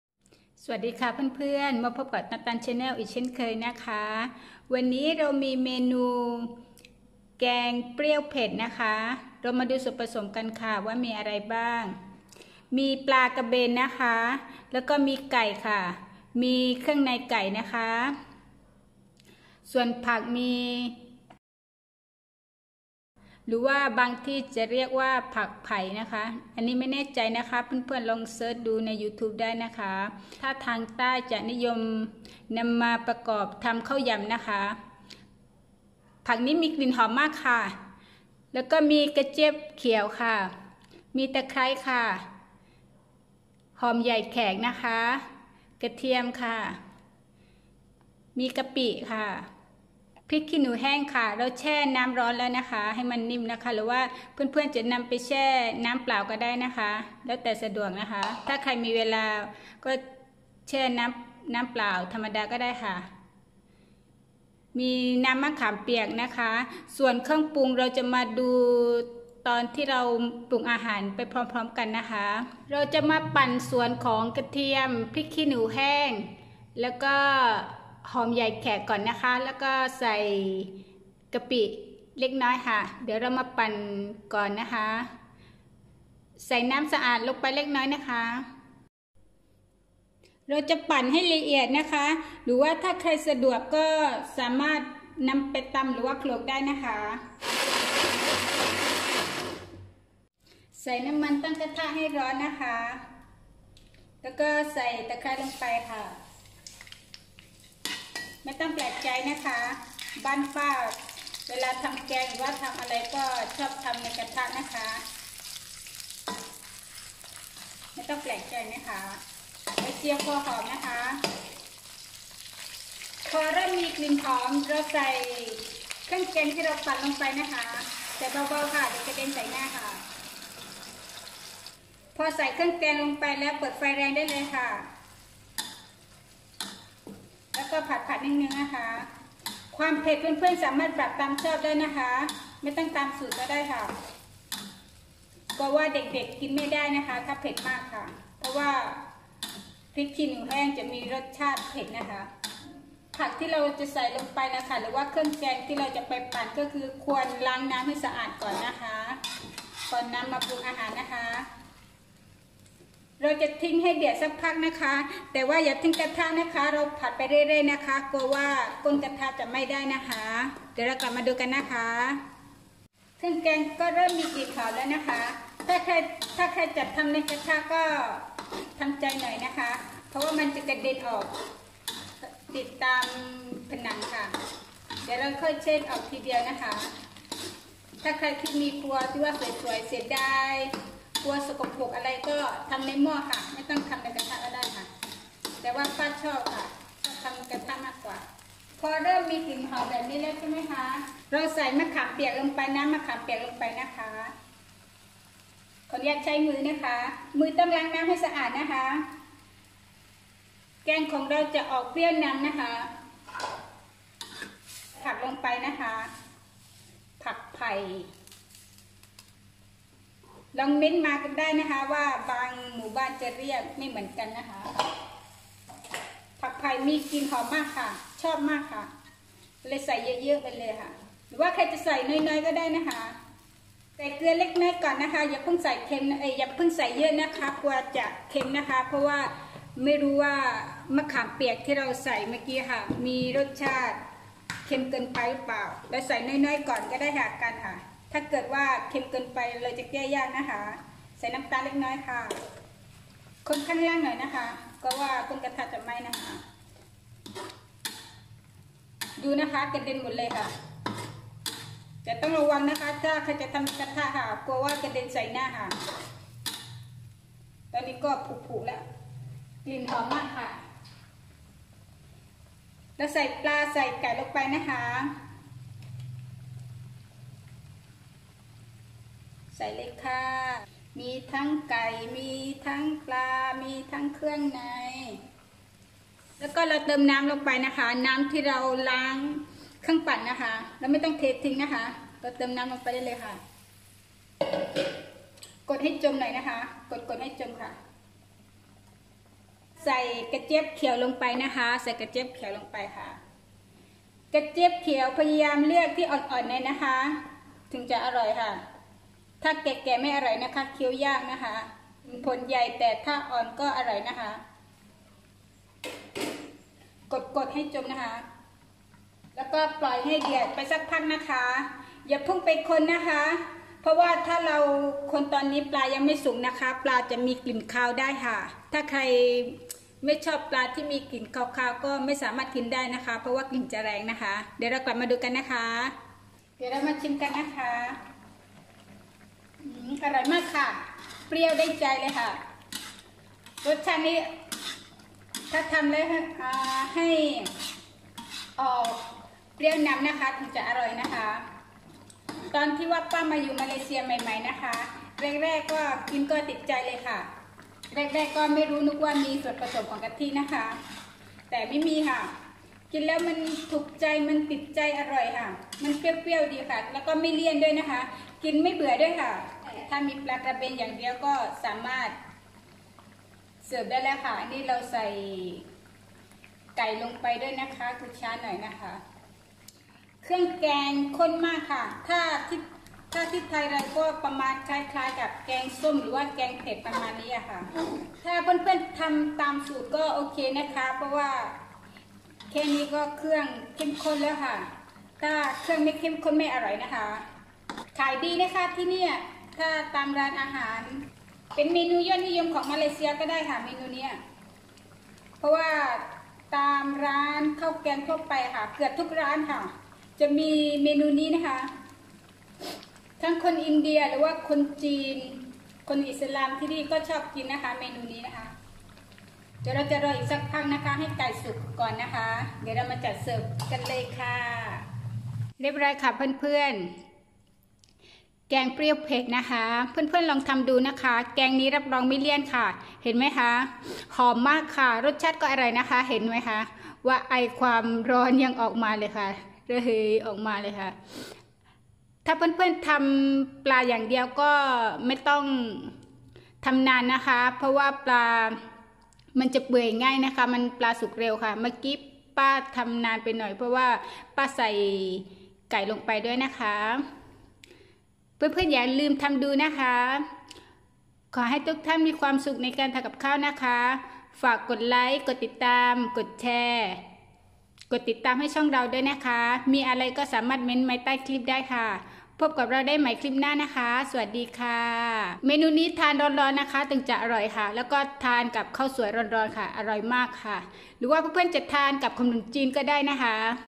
แกงส้มสูตรมาเลฯเปรี้ยวเผ็ด​ เครื่องแกงเข็มข้น​ อร่อยต้องบอกต่อ แม่บ้านต่างแดน​Ep. 37/ Tantan channel
แกงเปรี้ยวเผ็ด

1.ปลา​กระเบน​ 150​  กรัม
2.ไก่+ตับ​ 70​  กรัม
3.พริกขี้หนู​แห้ง​ 50​  กรัม
4.ขิง​ 10  กรัม
5.หอมใหญ่​แขก​ 70​  กรัม
6.กระเทียม​ 15​  กรัม
7.ตะไคร้​  1  ต้น
8.กะปิ​ 1/2  ช.ช
9.มะขามเปียก​ 100  กรัม
10.เกลือ​ 1/2  ช.ช
11.น้ำตาล​ทราย​ 1  ช.ช
12.น้ำมัน​พืช​ 1 ช.ต
13.น้ำสะอาด​  300​ ml
14.กระเจี๊ยบ​เขียว​ 120  กรัม
15.ผักแพว​ 120  กรัม

แกงส้ม แกงส้มสูตรมาเล​ฯ# Tantanchannel